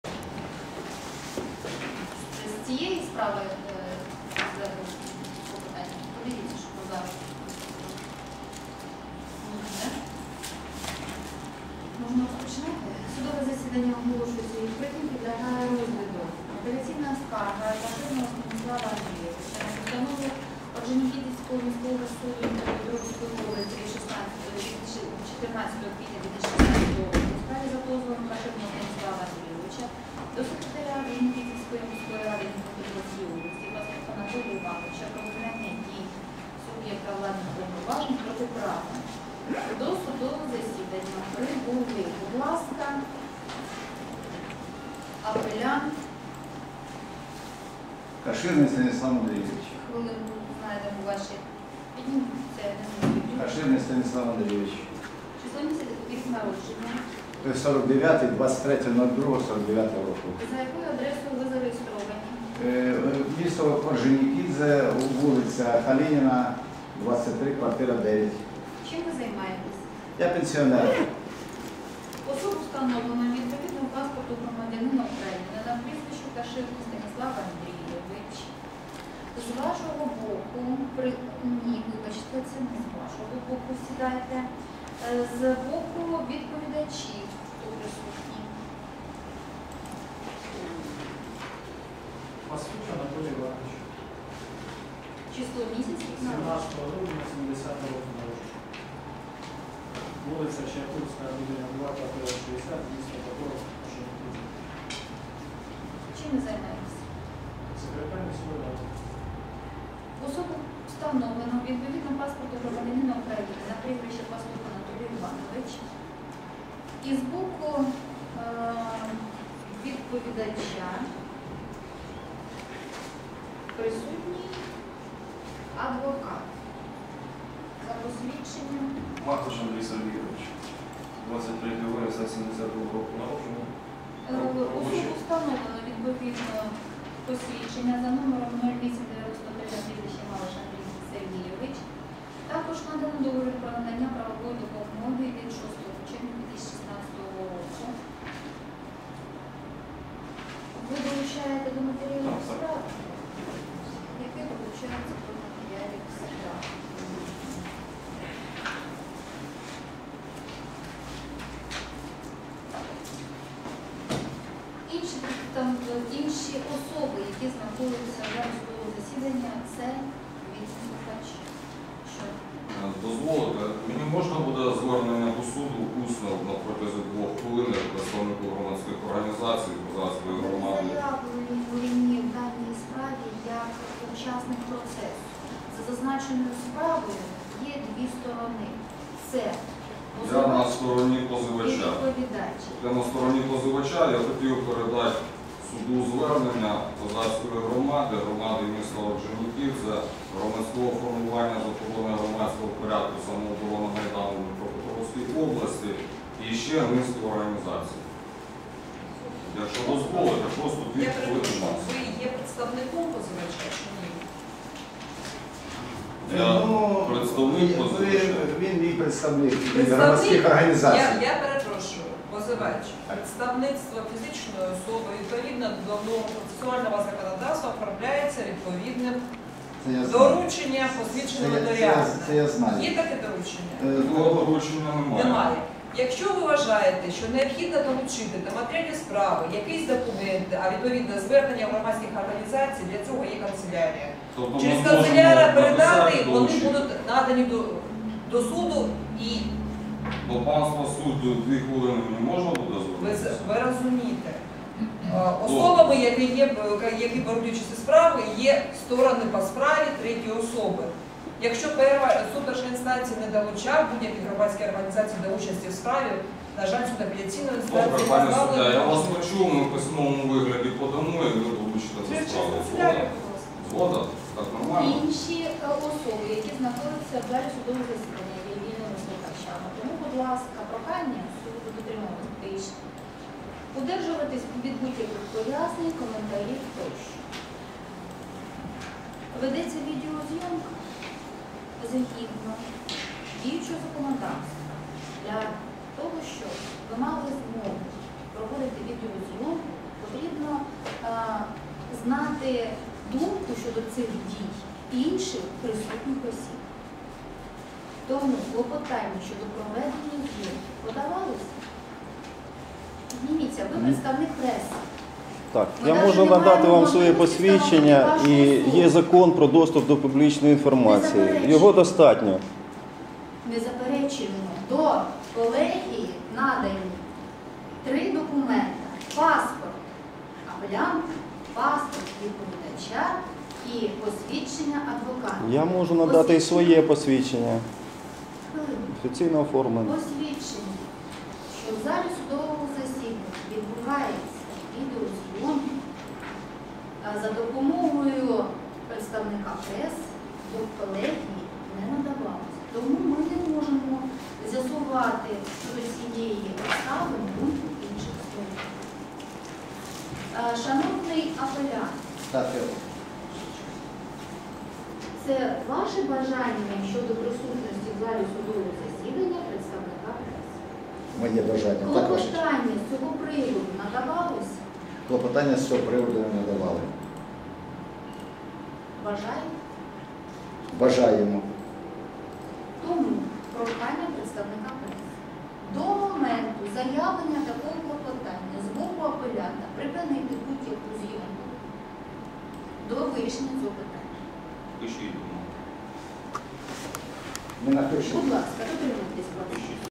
БИТВА ЗА ВЕЛИКИЙ ЛУГ
Сьогодні відбулось засідання апеляційного суду з приводу незаконних дій секретаря міськради Орджонікідзе, коли він після закінчення терміну даного на переіменуваннфя самовільно подав пропозийію начебто від мешканцев міста назвати місто москальською назвою Покров, зхнехтувавши іншою назвою, що є історично обгрунотваною мешканцями та Інститутом національної пам'яті  і підтриманою на ВЕЛИКИЙ ЛУГ. Рішення апеляційного суду - визнати всі позивні вимоги пана Каширного з правом оскарження відповідачем впродовж 20 днів до Вищого адмінсуду...